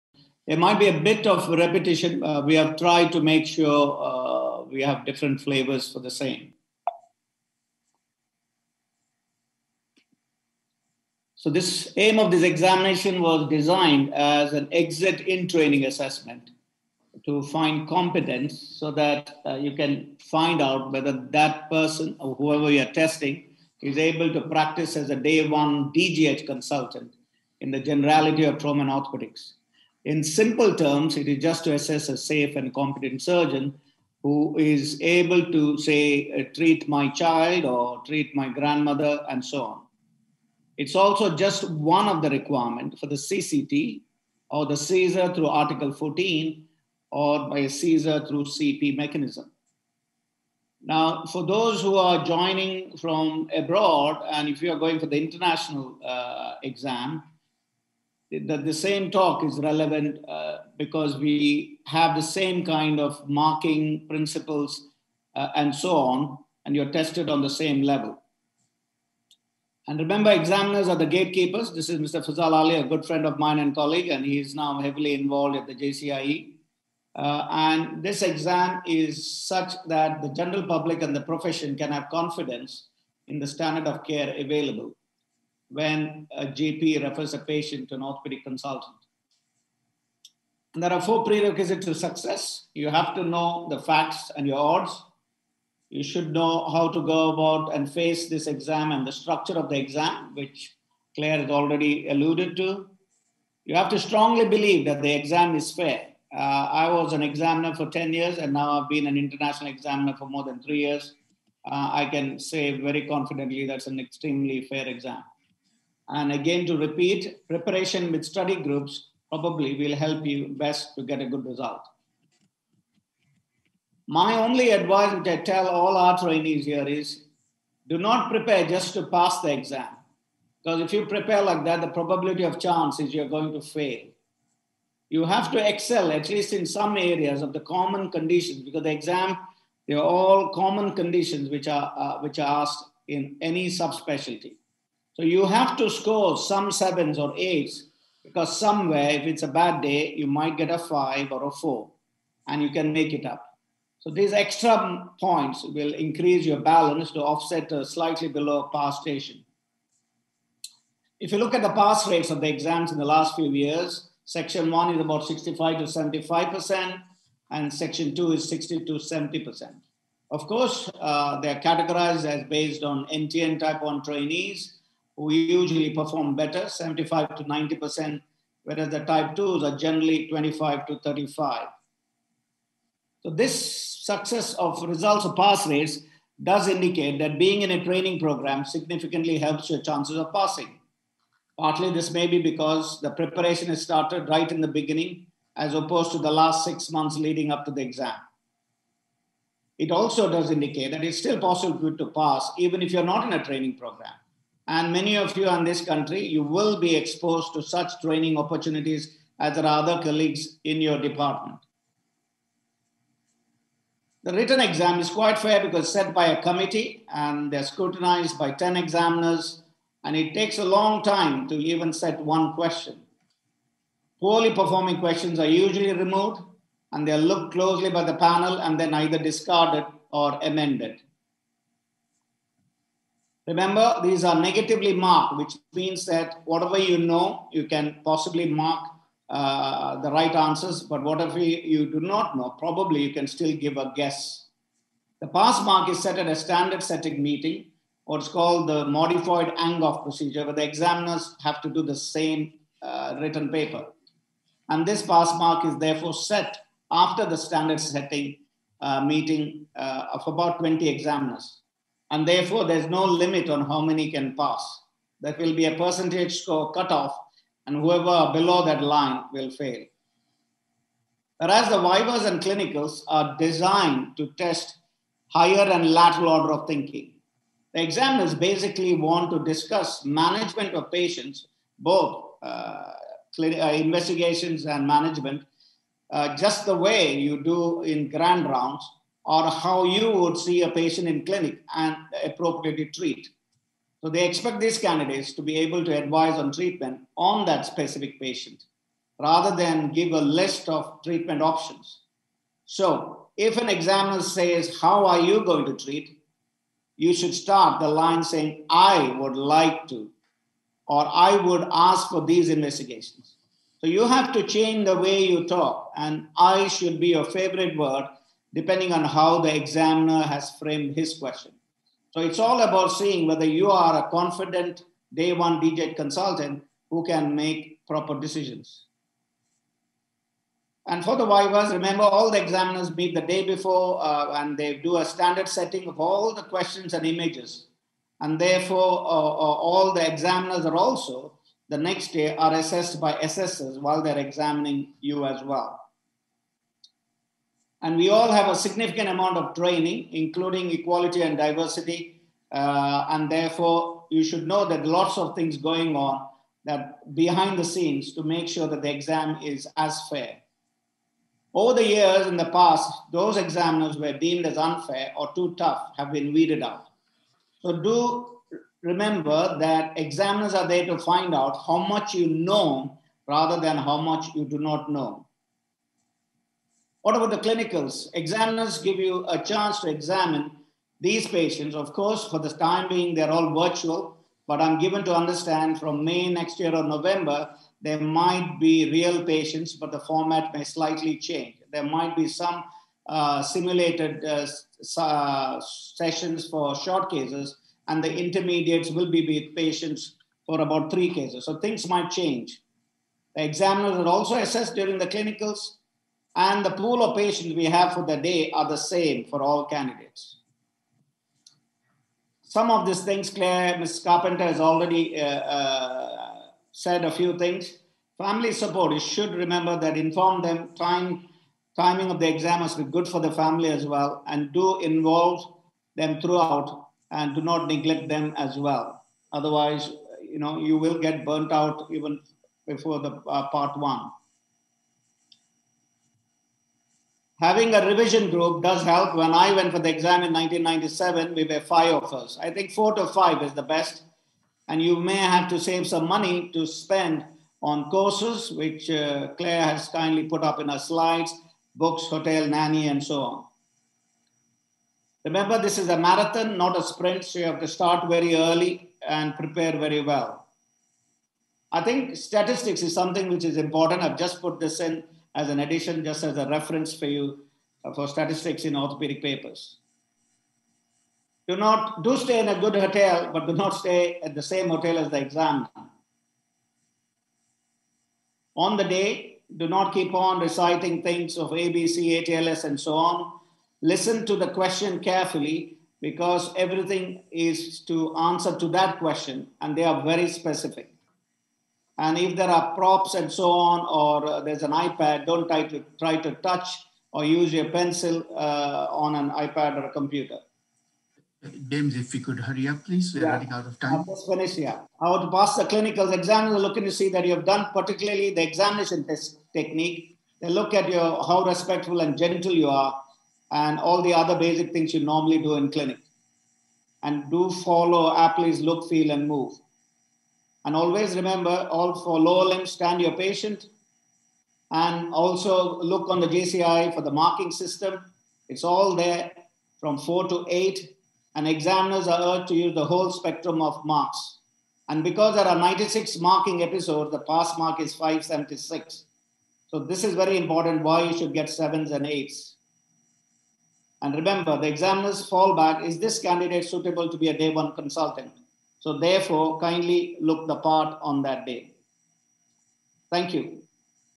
How to Pass FRCS Ortho Exams - JCIE & JSCFE - Advice from a FRCS Examiner (UK & International FRCS)
To obtain a CPD certificate for attending this lecture,

This video provides a roadmap for success, including:
Key study resources and strategies
Maximizing revision efficiency
Effective exam techniques
Top tips from experienced surgeons